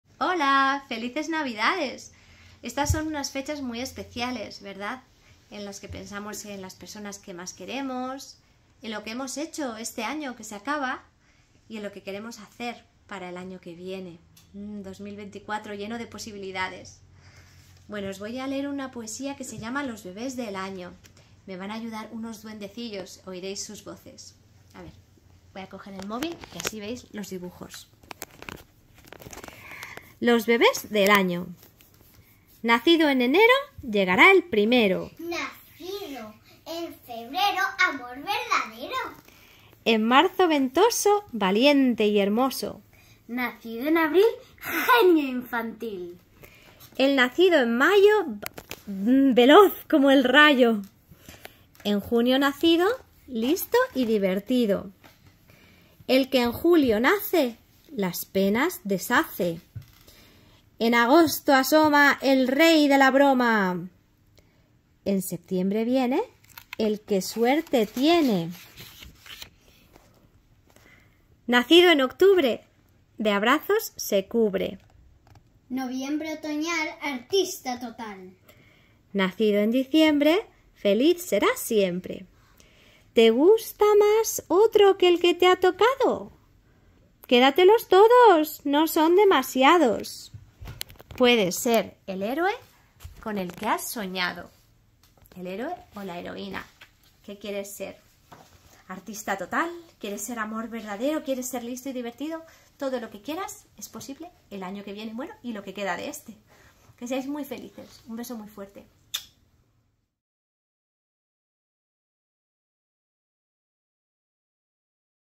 Especial Navidad: "Los bebés del año"
Y nada mejor para empezar el año 2024 que un poema de Vanesa, acompañada de sus pequeños y adorables duendes. ¿Quieres saber cómo eres por el mes en el que has nacido? Pues escucha atentamente y lo sabrás.
A mí me encanta el que me ha tocado.
¡Feliz día de Año Nuevo!